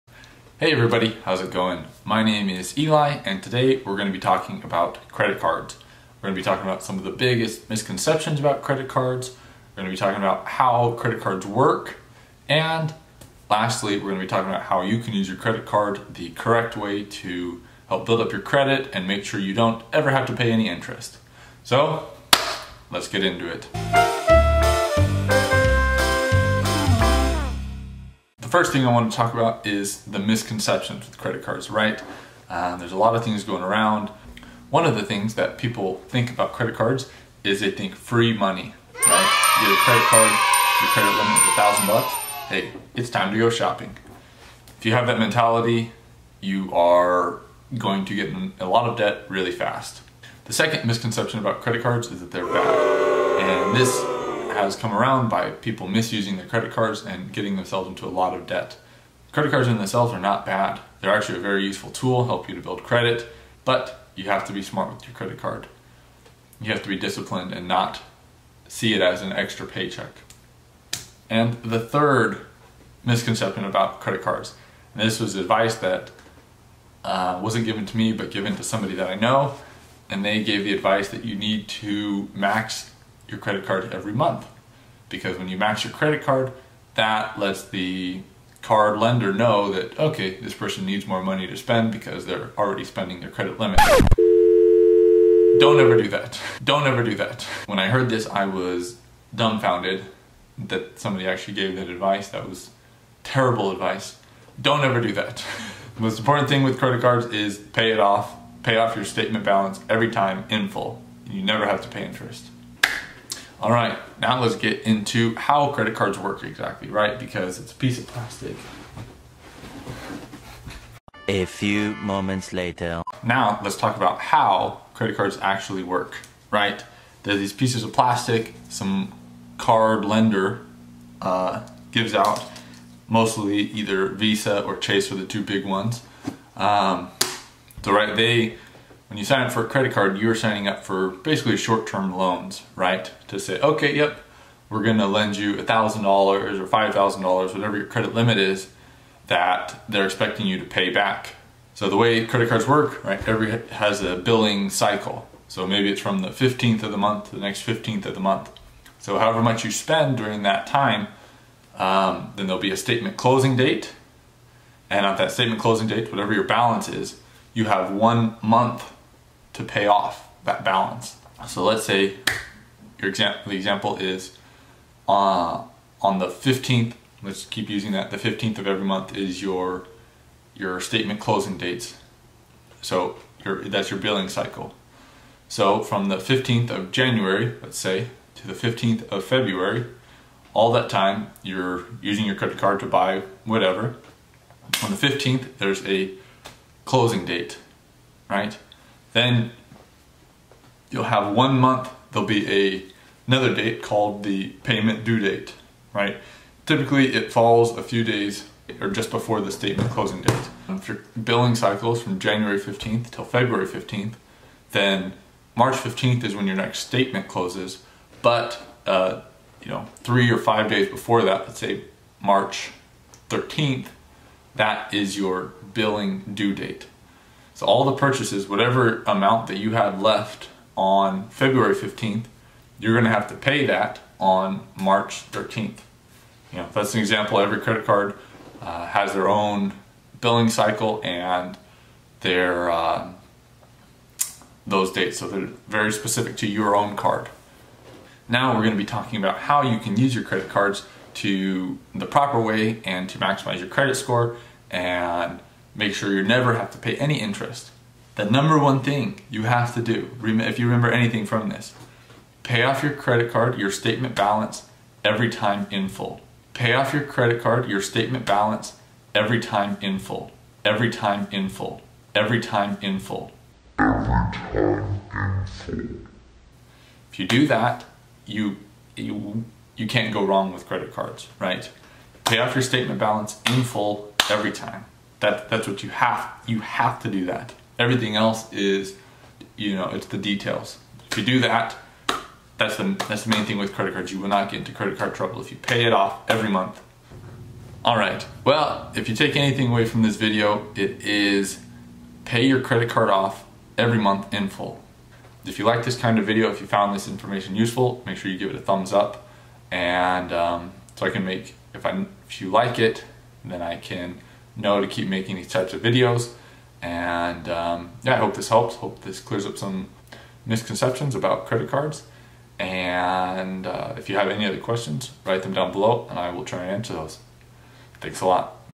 Money Talk - Credit Cards Basics - How to not pay any interest!!!
This video talks about credit cards and some of the biggest misconceptions about them. I was planning on going more in-depth, but decided that it would be best to make a separate video detailing statement closing dates, payment due dates, credit utilization, and billing cycles.

Rich Dad Poor Dad